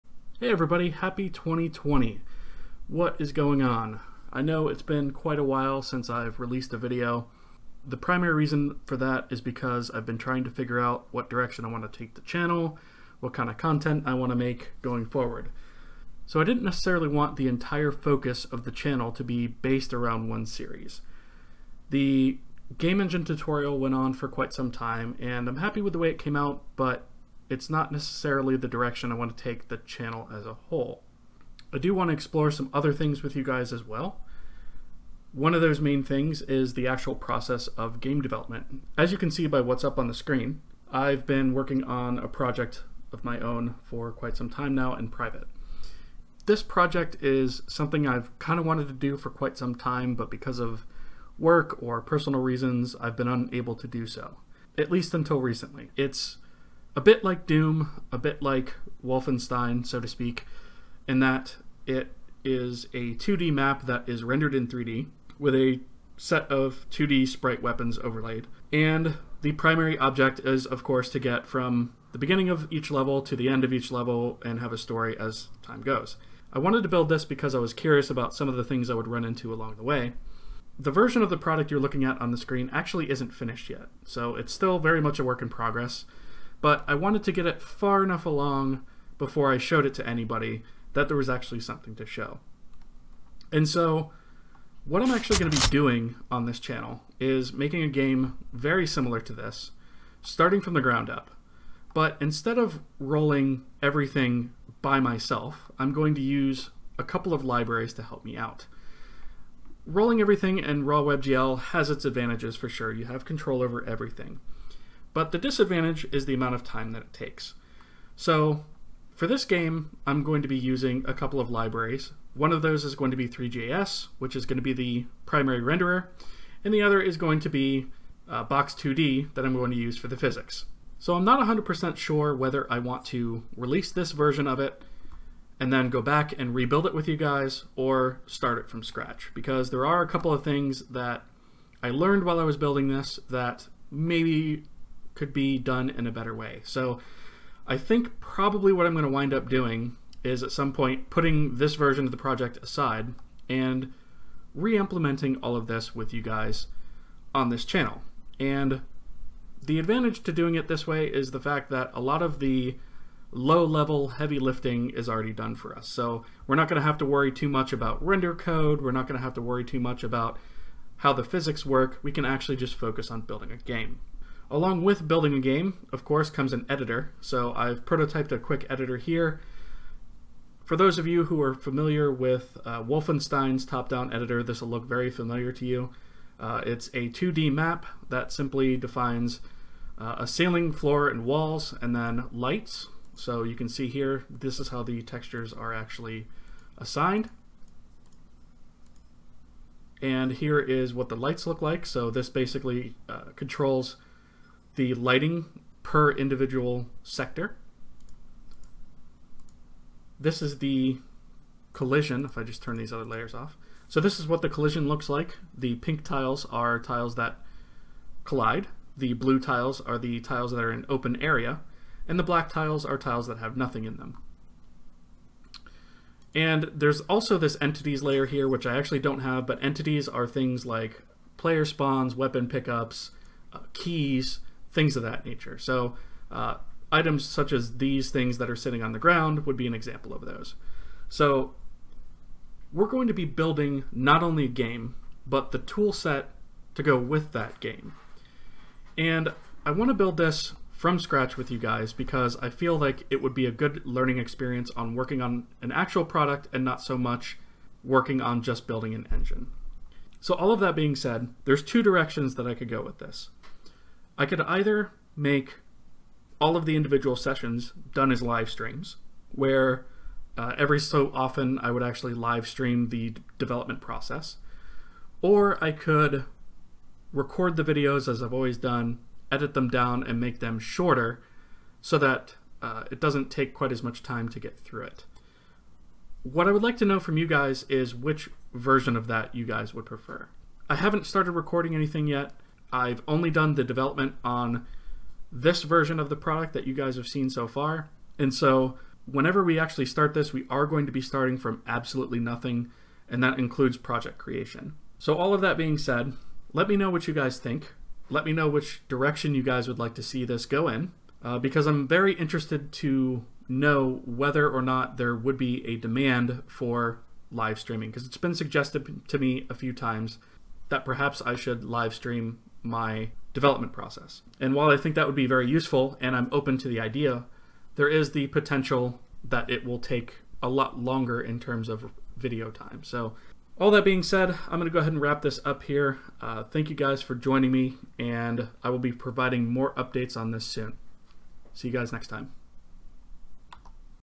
GLOOM Series #1 - Intro (TypeScript Game Dev Series)
This video is the first in a new series where we will be creating a game together, from scratch to completion using only a couple libraries for expediency.